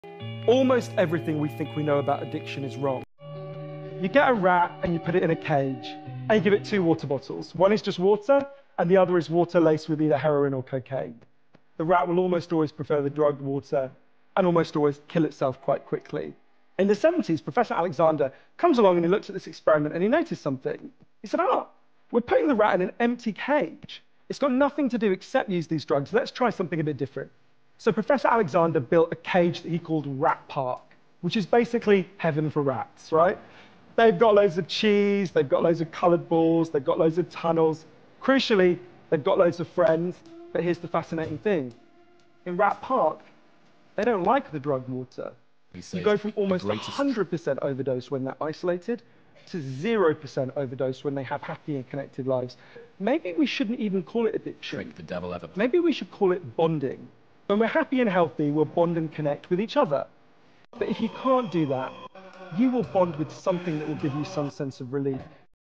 This VIRAL Rat Explains Addiction…🤯🧠
The famous "Rat Park" experiment completely changed our understanding of addiction. It revealed that when a rat is isolated and alone, it will almost always choose dr*gs. But when it's given a happy life with friends and connection, everything changes. This is the profound truth about what really causes addiction. ⚡